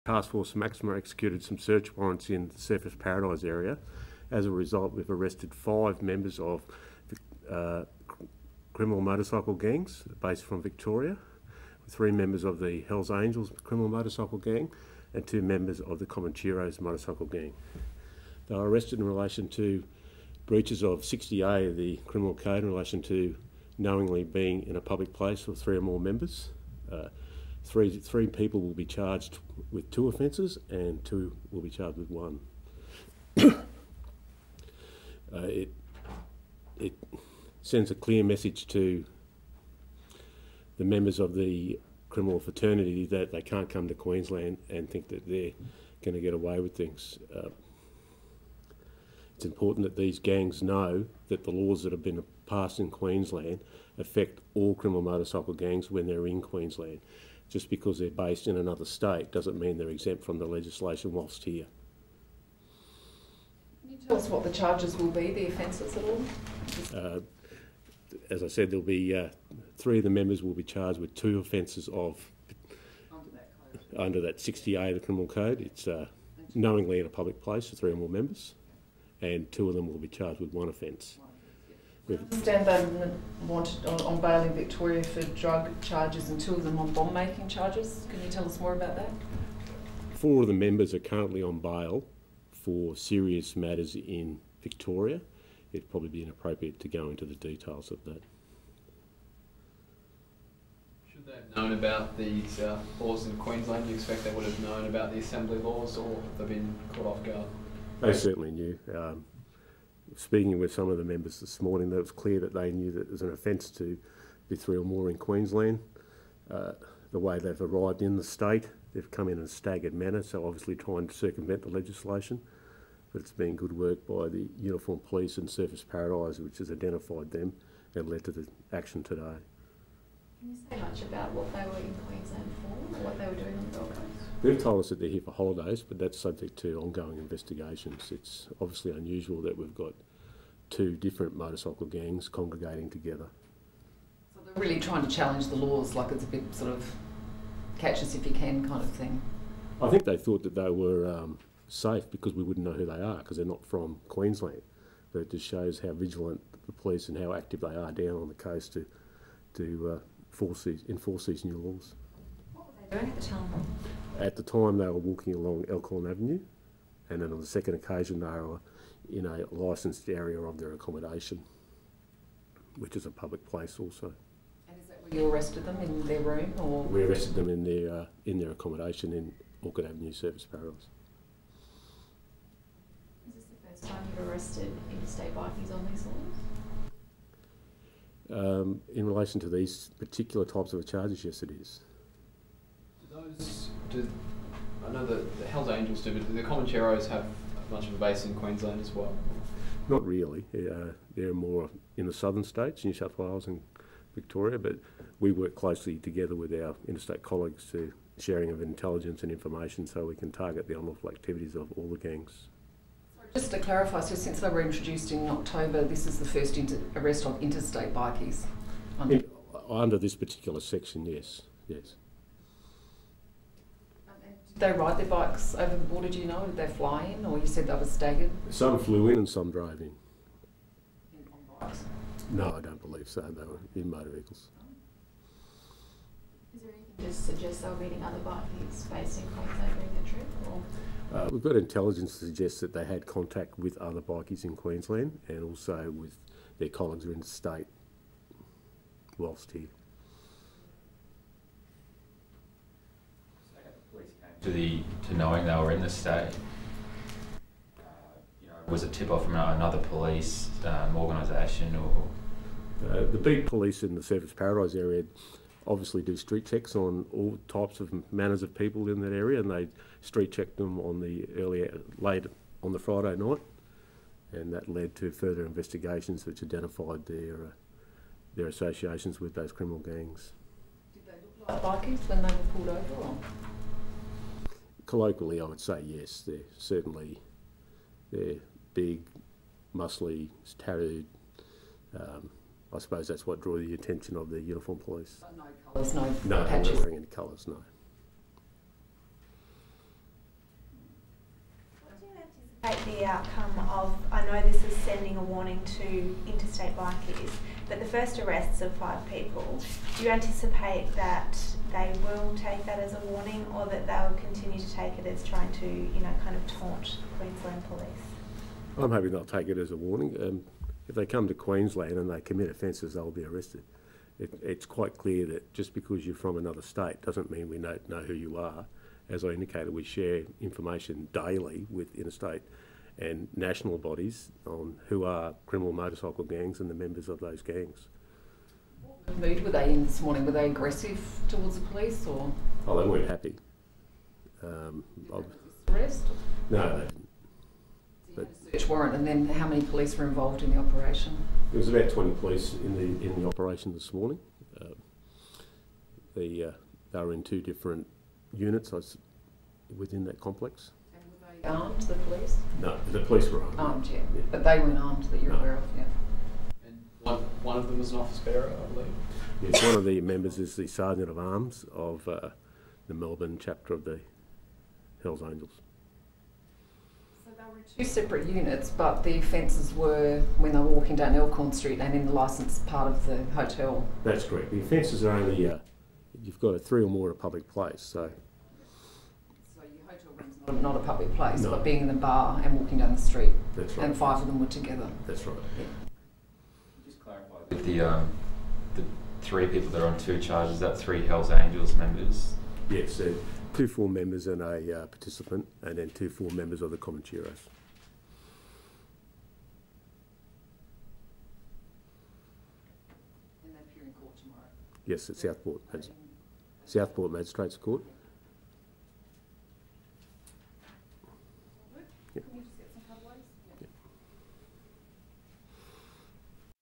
Taskforce Maxima CMG arrest Gold Coast January 5, 2014
Detective Inspector Brendan Smith from Taskforce Maxima provides comment on arrests made on the Gold Coast